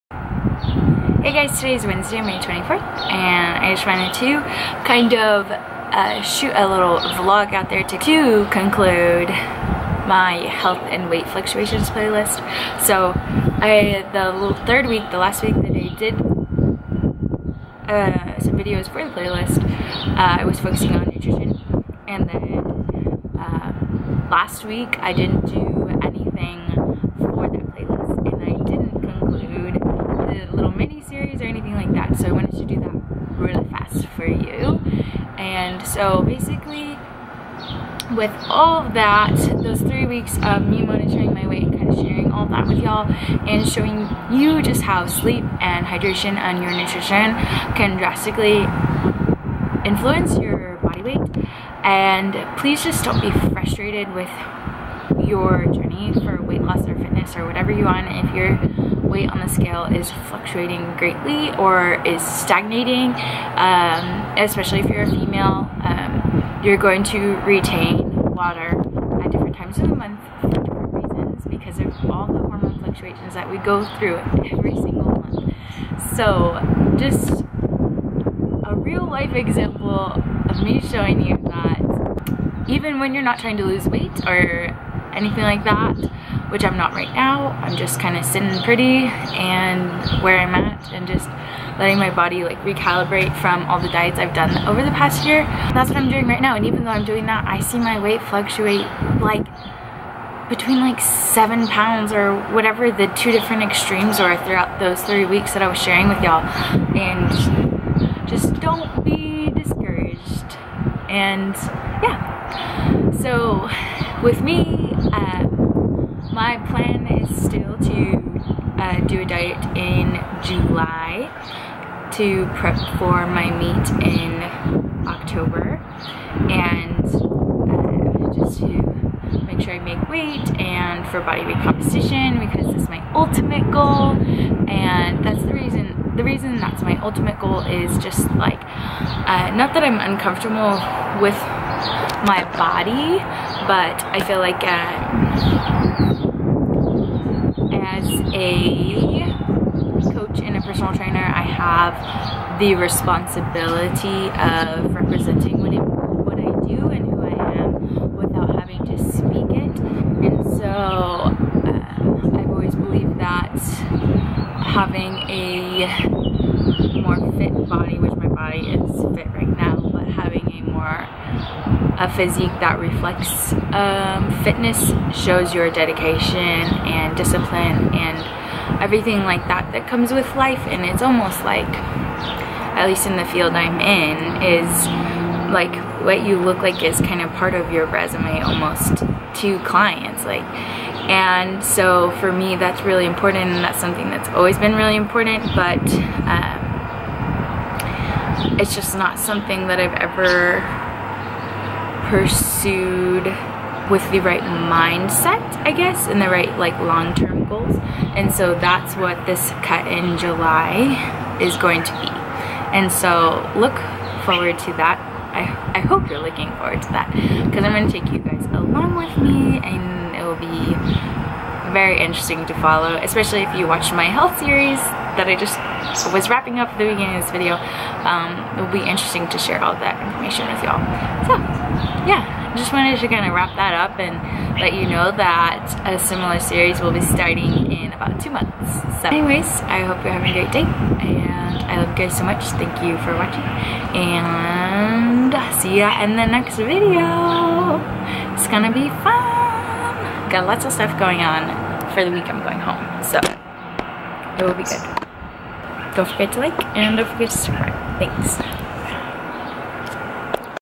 health and weight fluctuations conclusion
In conclusion of my three week weight fluctuation series, I wrap it up and let you in on what is going on now and coming up soon. I hope the discussion in this video is relatable to you even if you do not work in the fitness industry. I think all of us hold ourselves and our bodies to a higher "standard" than our current state, no matter if we reach our goals or not, we move the goal post a little farther each time we do. Looking forward to the next series regarding this subject! I hope you are too!

Shawnee